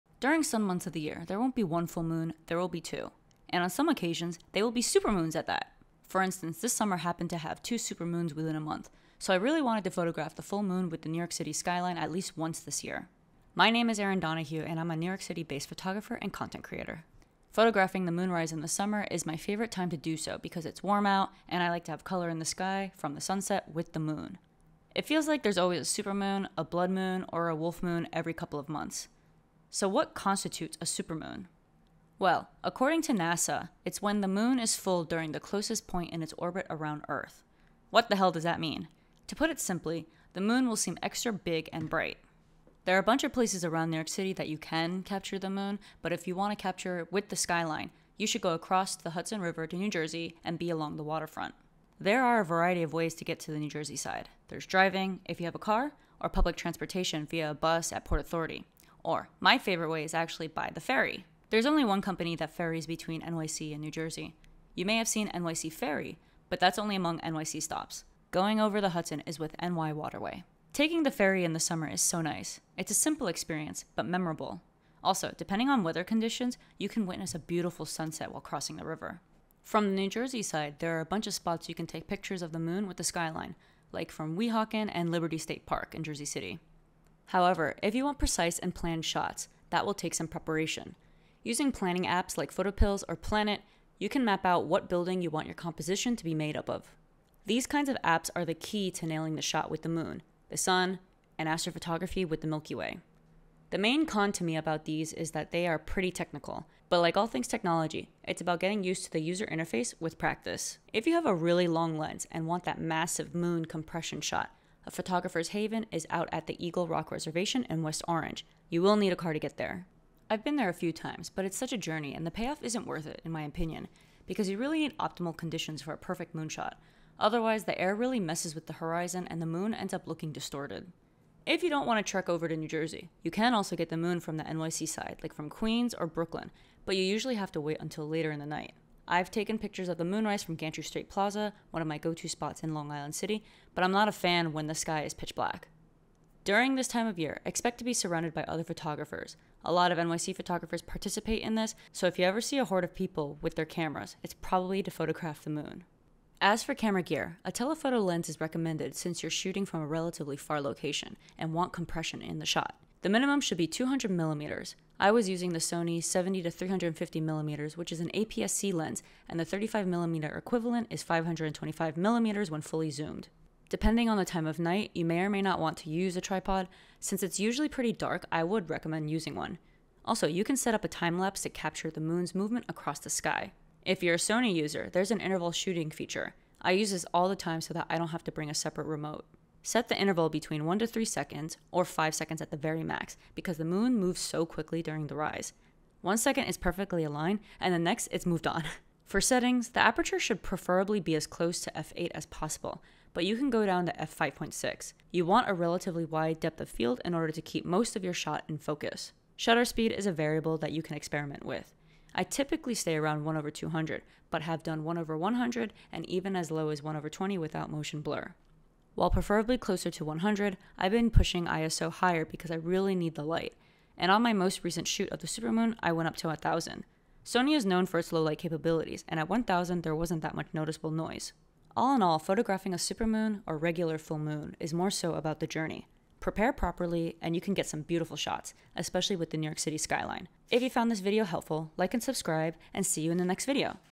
How to Photograph a Supermoon With The NYC Skyline
Capturing full moons like the supermoon isn't hard, it just takes a bit of planning and practice.

I go over some preferred locations, camera gear, and settings in order to nail the shot every time!

+All camera gear I use